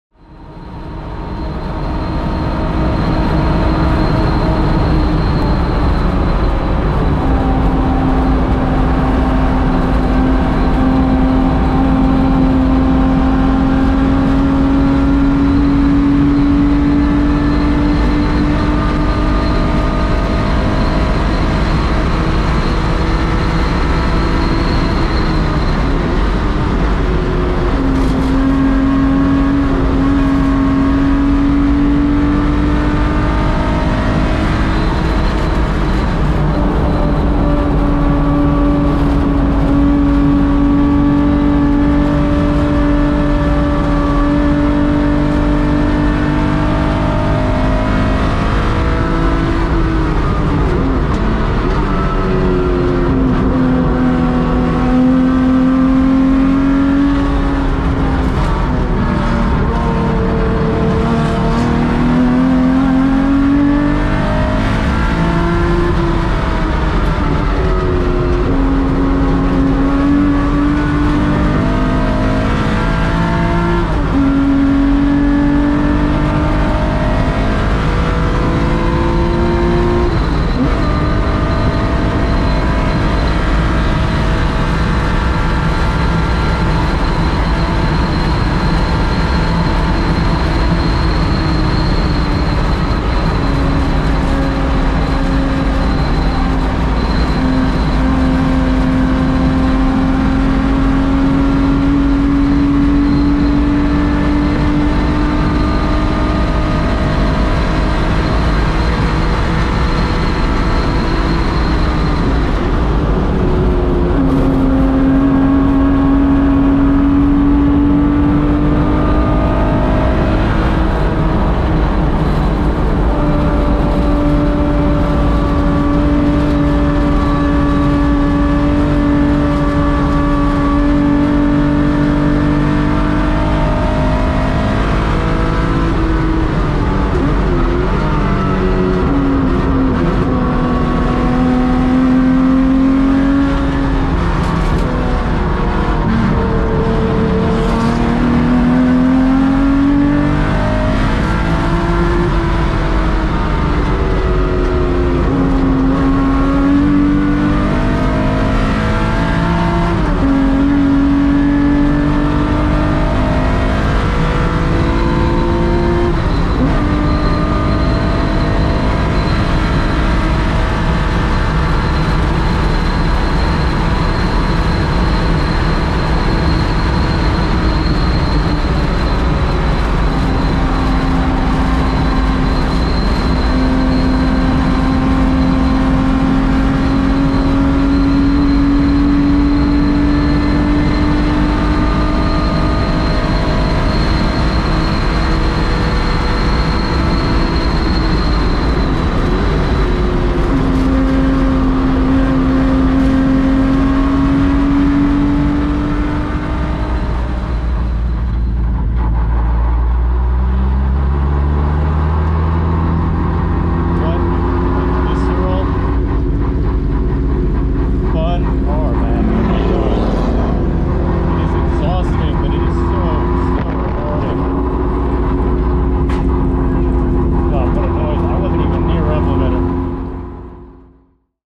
DRIVER'S EYE: 3.4L Aircooled 911 Race Car at Willow Springs | May 2021
Hi everyone! Here's is a couple fast, bumpy Willow Springs laps in a race-prepared '86 Porsche 911 built to POC GT3 regulations. Filmed during open practice for POC's Tribute to Le Mans weekend as I set reference data and video to coach with.

This is likely the highest average speed I've sustained in a single lap, don't think it dropped under 90mph very much! No power steering, power brakes, driver aids, etc. 10 laps began to have similar fatigue levels to a race! So, so enjoyable to put on the limit with no artificial ties between you and the machine. Easily the most fun racing car I've had the fortune to lap this year! While these laps had their flaws and I have plenty still to learn with old 911's, it gives a good perspective on what it was like (apart from the arm fatigue).

Again a unique experience to learn rear-engine dynamics of old 911's especially around a circuit this quick, although the aerodynamic modifications and widened stance help this 911 retain a less aggressive weight balance. These tires were far from new, however we added more fuel to the front tank to give the front more bite. Hopefully I can find my way into racing one of these someday!